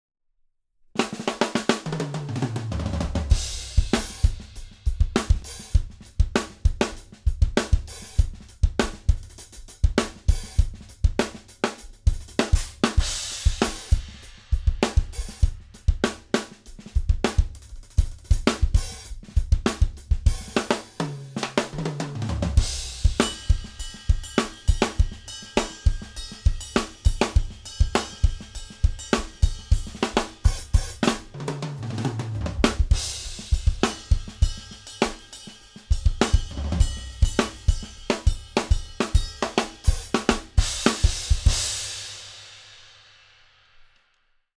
Drum Mic Test w/ Four Mics: Overheads/Snare/Kick
My very first attempting recording and mixing live drums.  Much more room for improvement obviously.

Mics used in the video:
• Overheads: Audio Technica AT2020 (XY config)
• Snare: Audix i5
• Kick: Sennheiser e602

All plugged into a Focusrite Scarlett 18i20 going into GarageBand